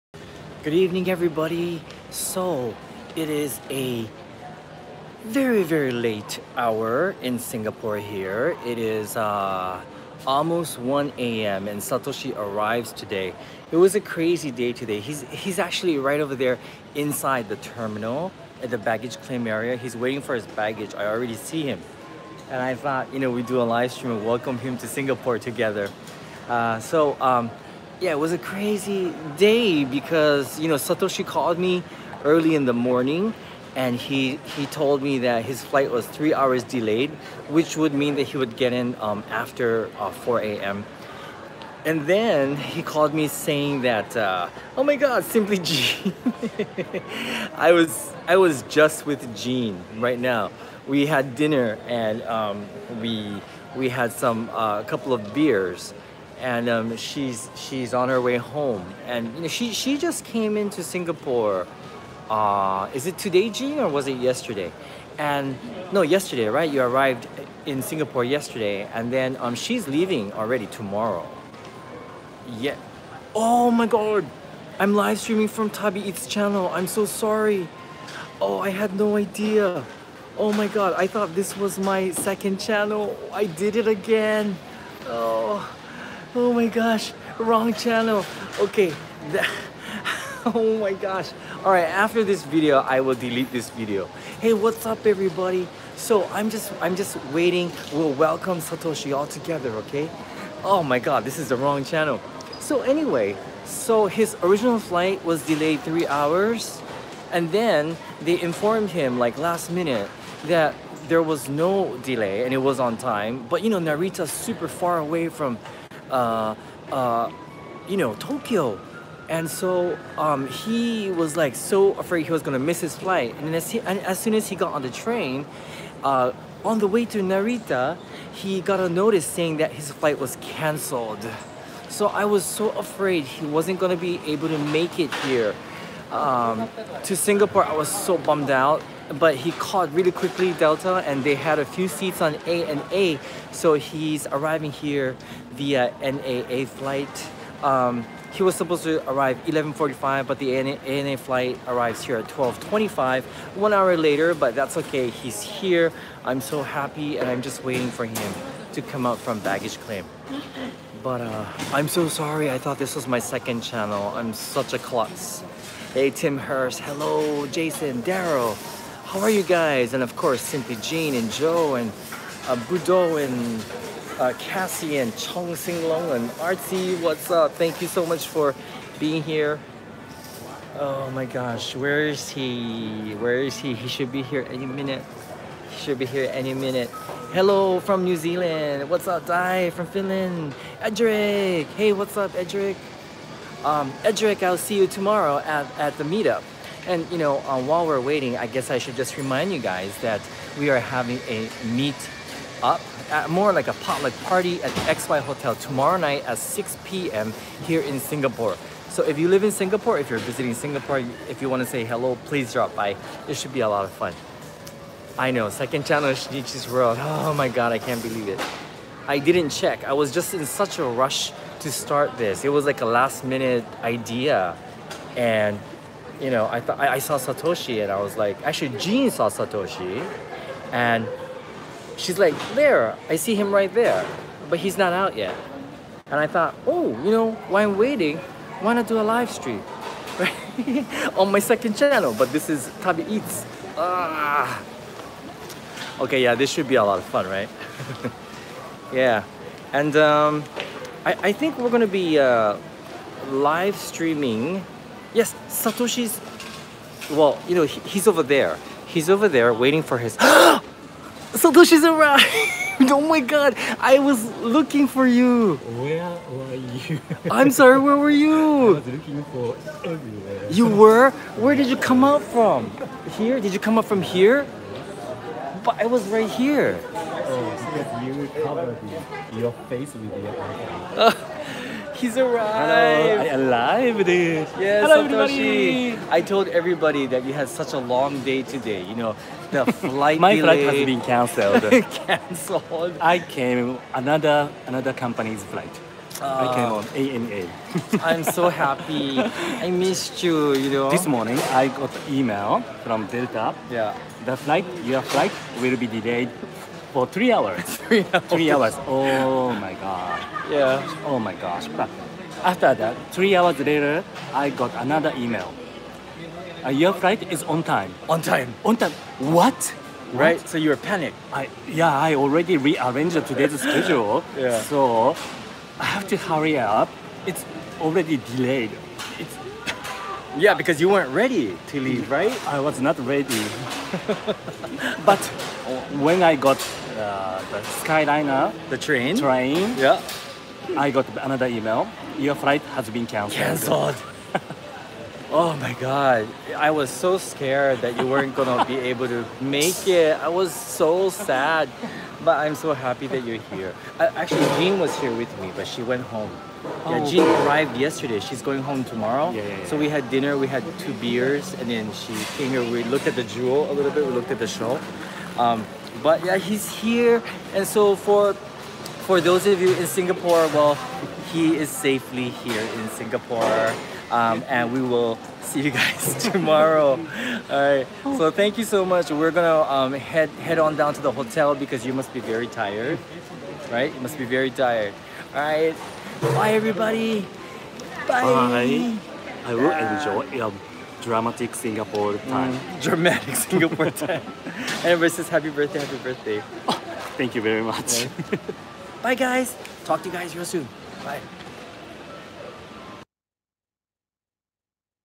Satoshi Arrives at the Airport
I'm going to pick Satoshi up at Changi Airport. So happy he is here!